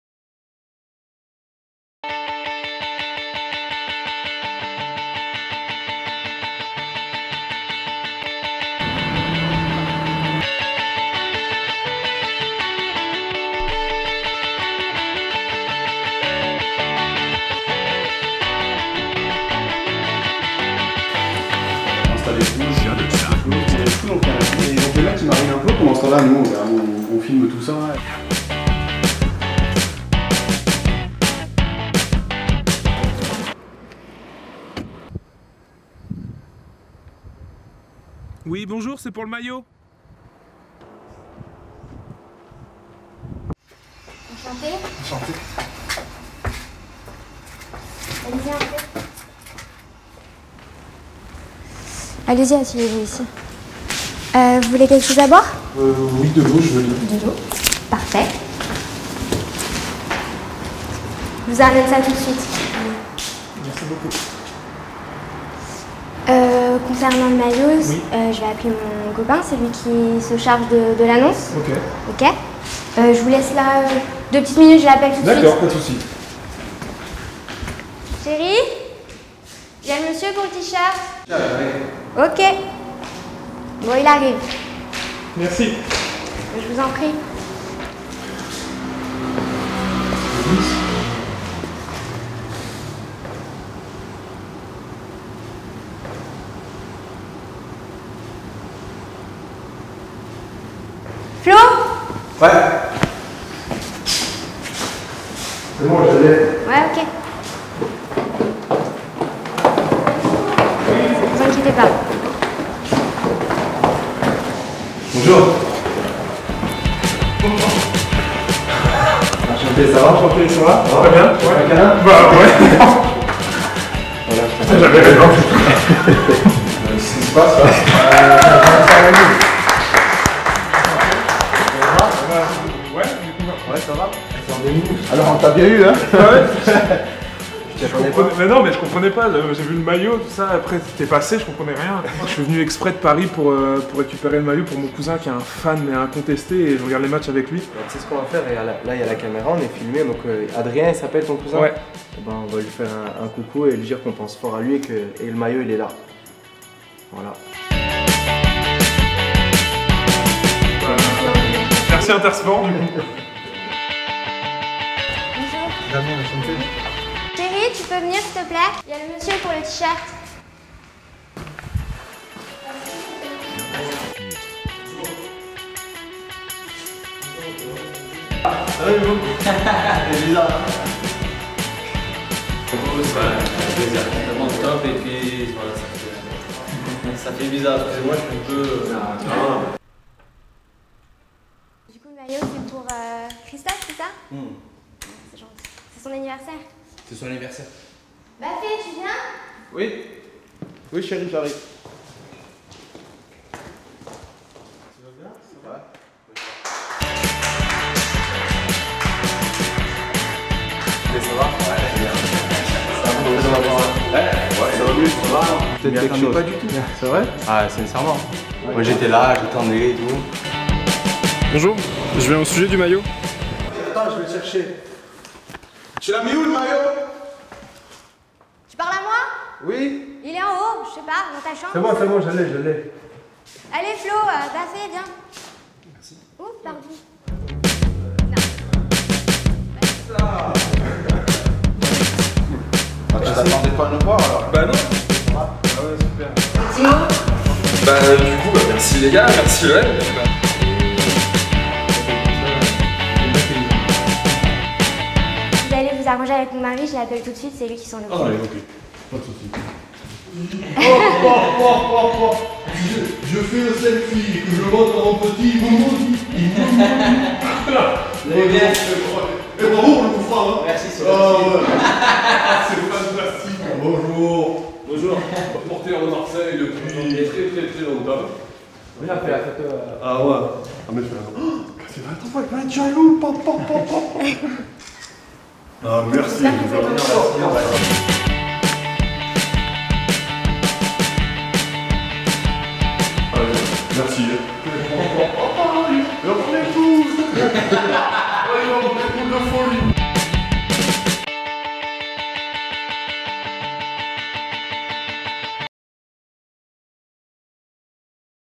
INTERSPORT piège les supporters de l’OM avec la complicité des joueurs !
A l’occasion d’une caméra cachée, le  partenaire principal de l’Olympique de Marseille INTERSPORT, a tendu un « piège » aux fans du club.
Après avoir répondu à une petite annonce concernant la vente d’un maillot dédicacé par les joueurs de l’OM, les acheteurs se présentent au lieu de rendez-vous fourni par un complice. Une fois arrivés sur place, les fans n’imaginaient pas se voir remettre le maillot par… Rémy Cabella, Bafé Gomis, Abou Diaby, Bouna Sarr ou encore Florian Thauvin !

Une surprise qu’ils ne sont pas prêts d’oublier !